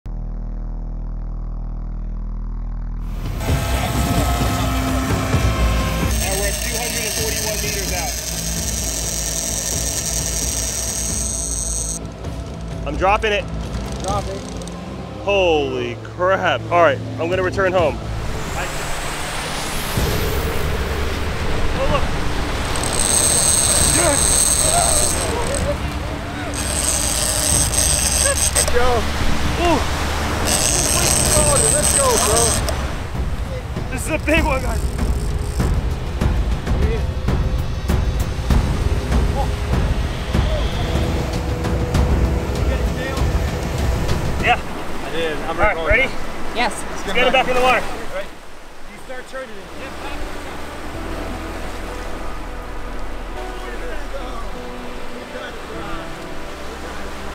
dropping BIG BAITS into the ocean
beach fishing is always interesting because you never know what will hop on your line! this beautiful shark was safely released